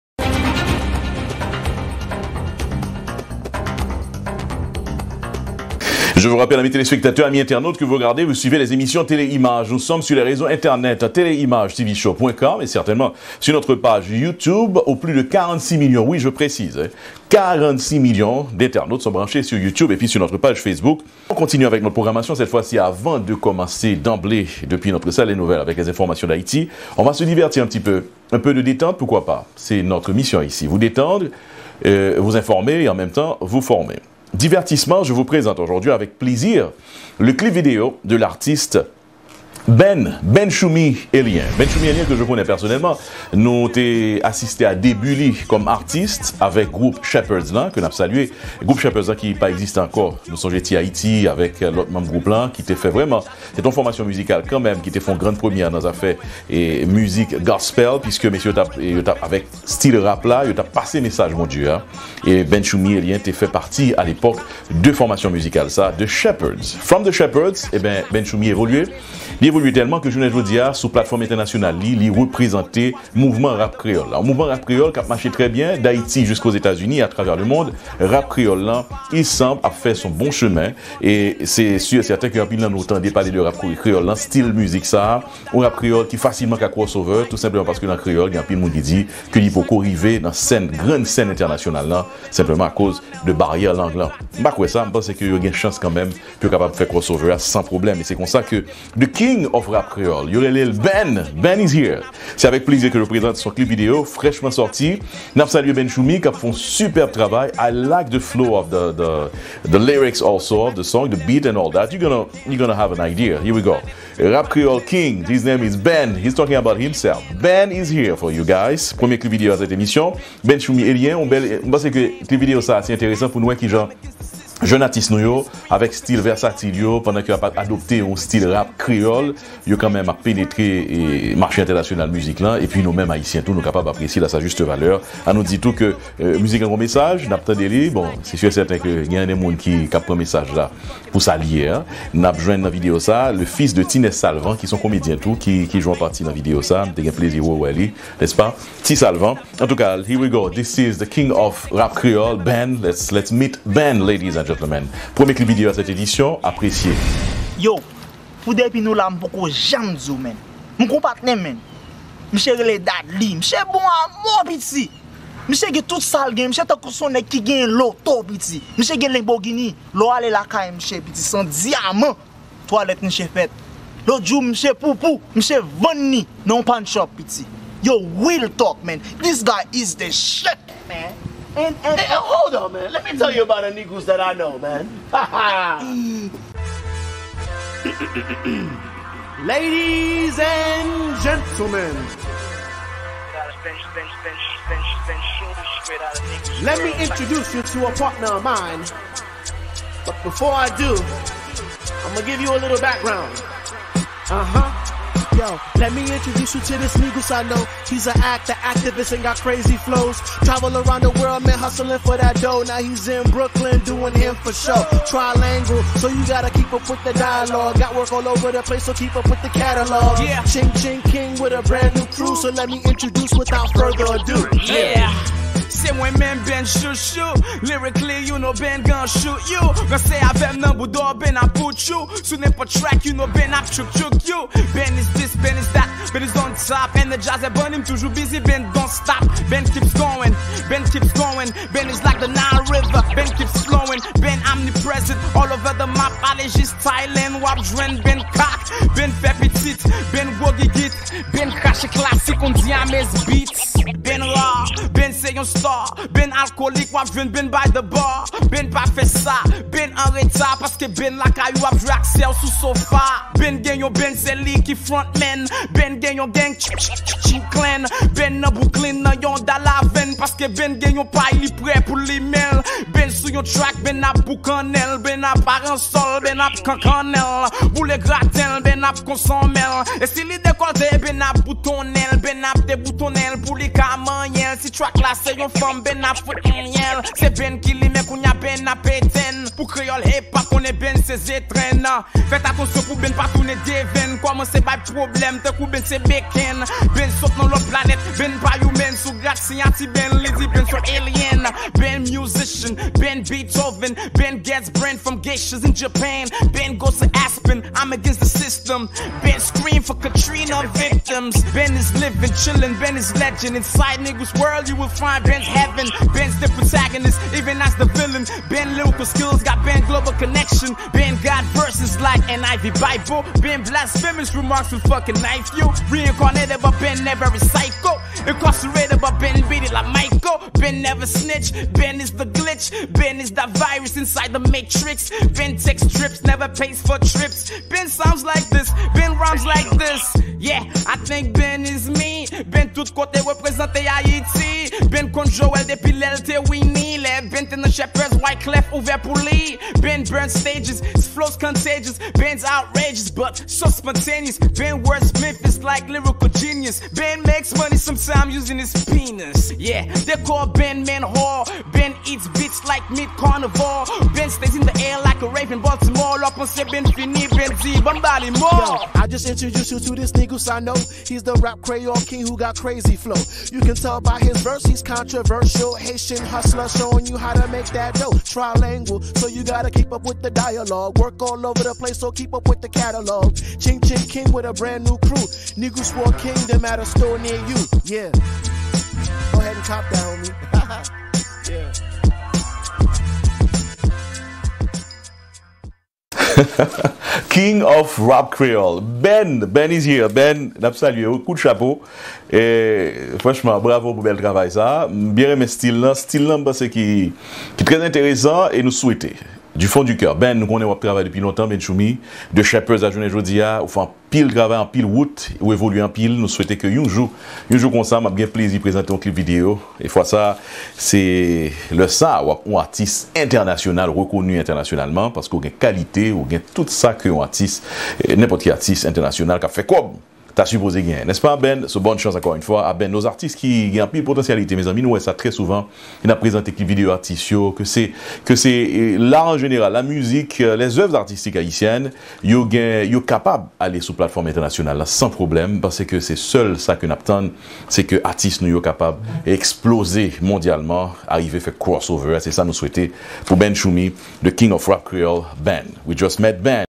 Rap Kreyol vs Rap State se menm kreyol la
MEET UP AK VALERIO - BENNCHOUMY (Bennchoumy Elien)
Valerio Saint Louis deklare Bennchoumy kom Roi / Wa / King of Rap Kreyol.